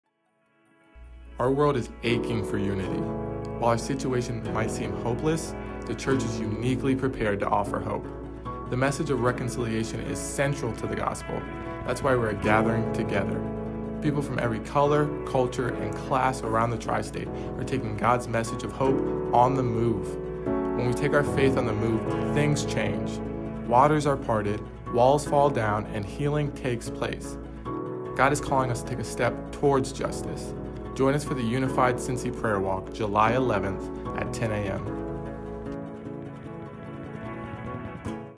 Unified Cincy: Prayer Walk | SOZO Loveland Church
If you have lived in this city for any time you know that we have continued to both battle, and make progress, on issues of "race". Like any city, we have had ups and downs, but I believe God continues to have His hand on it. You will also know that our church is an example of how healing can occur over time. This is a topic that we want to encourage others in by ourselves being a living example to our city.

This is why we believe a prayer walk in downtown Cincinnati, is the right next step. This week some Pastors met and committed to a show of unity around the power of God to heal through prayer. The problems we face are generational and stubborn but the solution will always involve Godly people praying for peace, justice, and reconciliation.

We would love for anyone in our church to join us this coming Saturday as we come together from 10AM to 12PM for a prayer walk from Yeatman’s Cove ( Next to Sawyer Point ) to Fountain Square. We will be one of more than 50 churches representing the gospel of Jesus. There is an opportunity for you to simply come or to volunteer.  If interested please let us know by RSVP.

If you cannot come, please join us by praying where you are!

God Bless you, for God called us to be His representatives of His redemptive power.

*** To RSVP for the event go to the Unified Cincy Prayer Walk Facebook event

 Something spiritual happens when you walk and pray. When you walk and pray, walls come down. When you walk and pray, waters are parted and new territory is taken. When you walk and pray, healing comes with every step of faith. Could it be that God is calling our church to take a step?

There is potential for thousands of people to show up and pray for unity within our community. I believe through the prayer of the saints, that any authority the enemy has over racism and bigotry will be crushed in Jesus Name!

SOZO Loveland Church
9781 Fields Ertel Rd
Loveland, OH 45140

Service Time: Sundays, 10AM - 12PM.